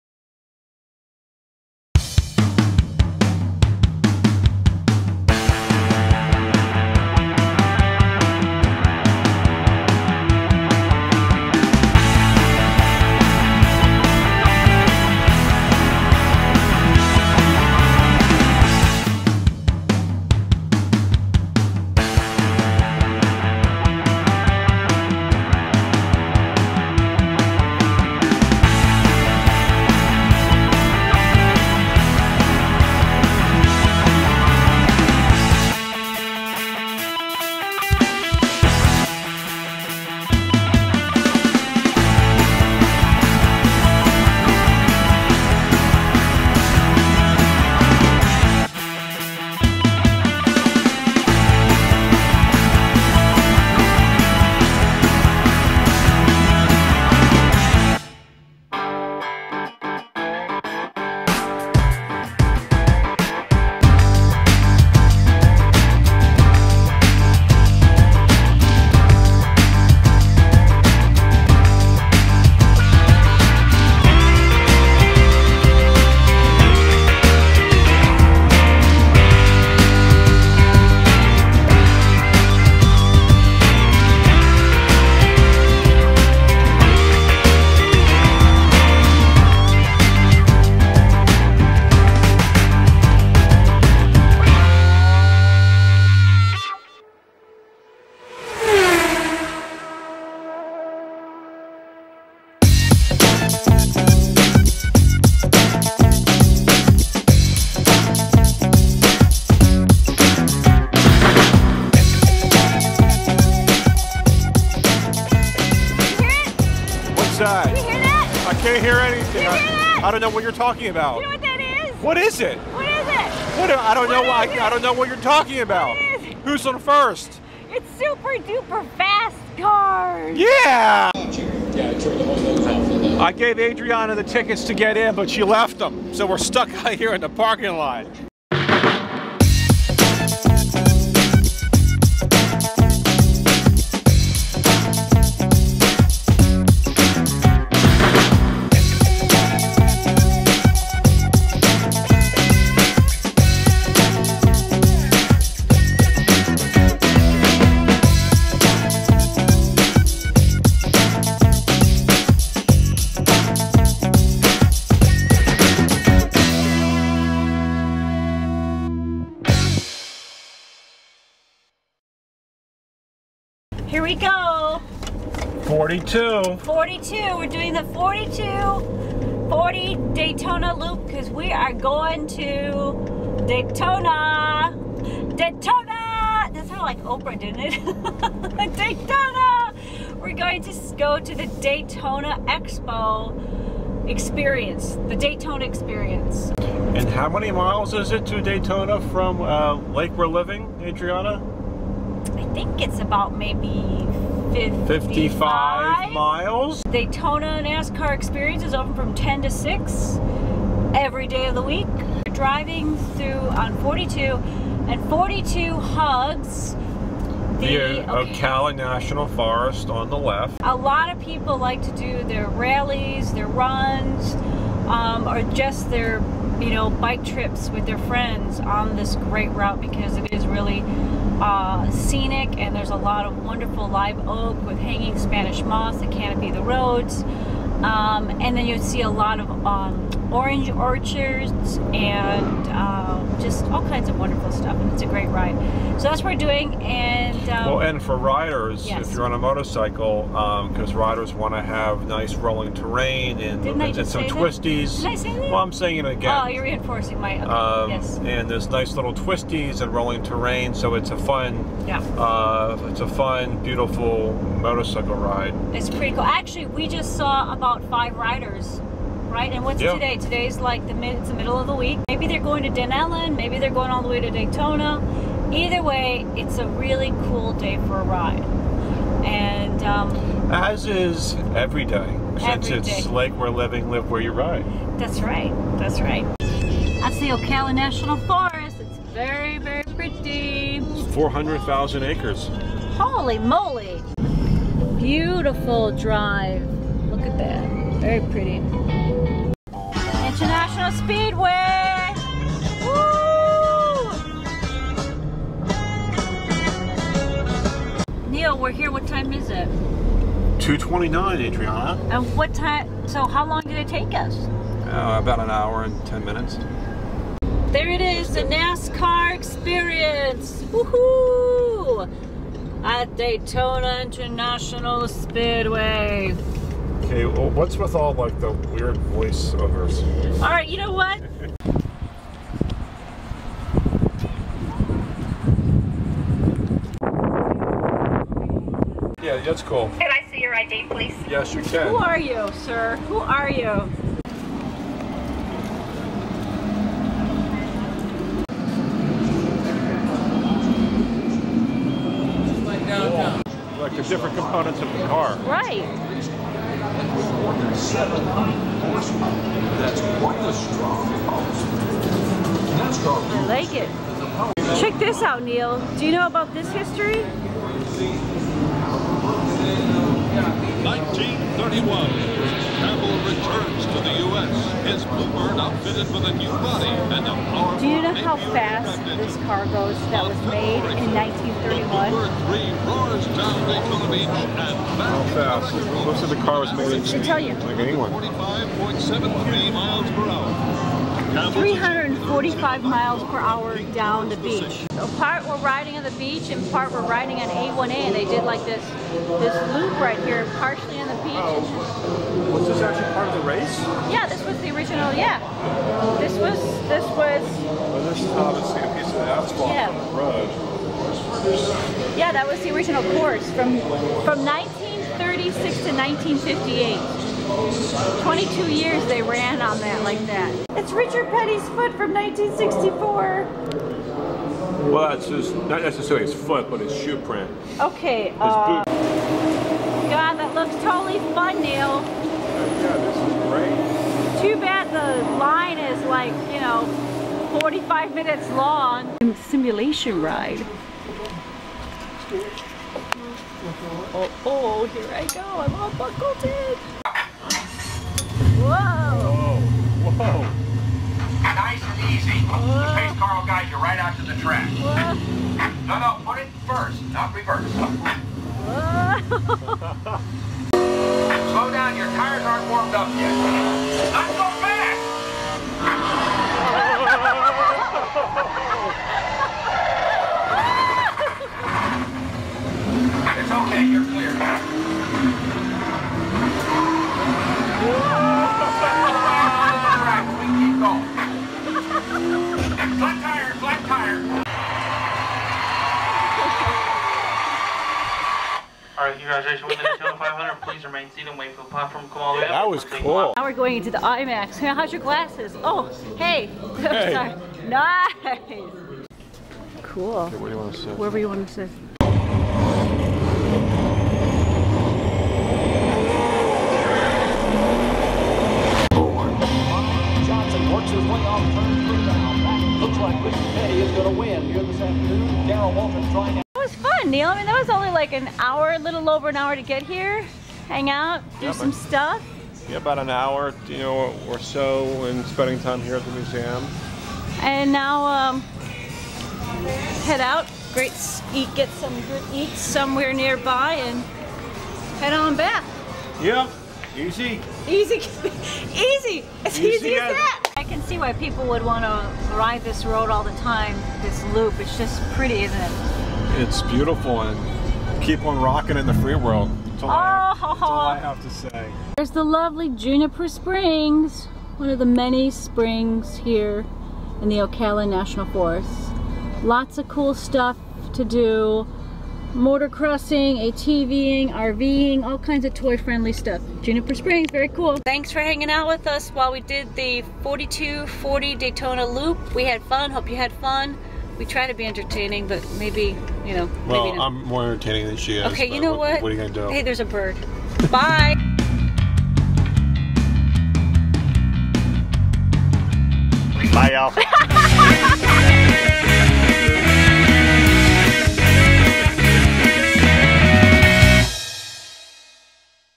Lake Weir Living: Webisode 178 "An Hour to Speedway!"
America's toy-friendly community is an hour to the Speedway...  Gearheads and Nascar Fans, this where you want to retire!  Be away from it all, and yet scenically close to the Daytona 500!

Lake Weir Living is a No HOA community.  Most retirement communities are homeowner association communities.  We are a boomer's best kept secret for an active, happy lifestyle.